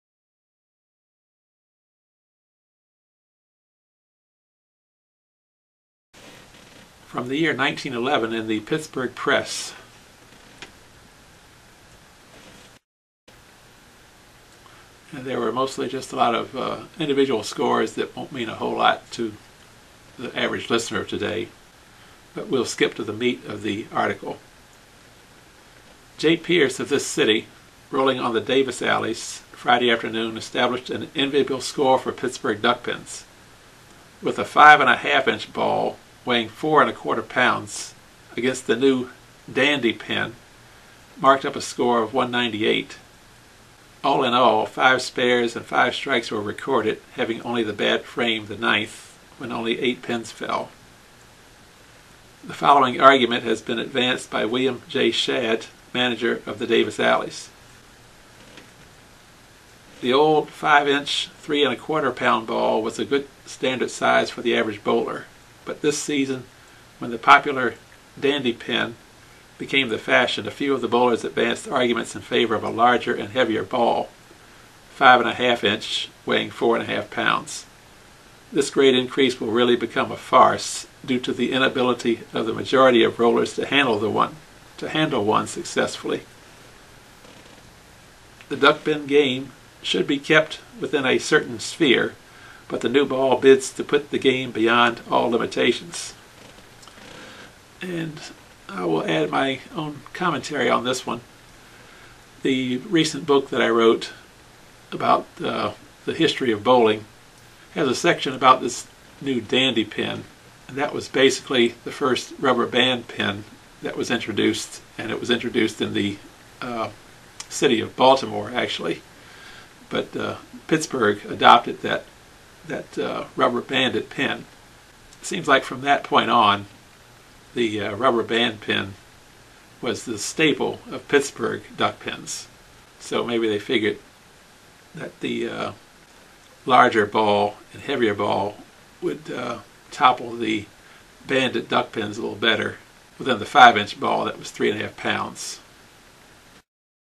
SpareTalk: Ep#10 - 10/29/1911 - The Introduction of the Rubberband Duckpin
Rod Weaver, Author of the book, "More Than Just a Game", highlights a few events that occurred, pre-21st Century on the date of October 29th in "SpareTalk: This Day in Bowling History" - Episode 10.

In this clip, the new Rubberband Duckpin branches off from the standard wooden pin game.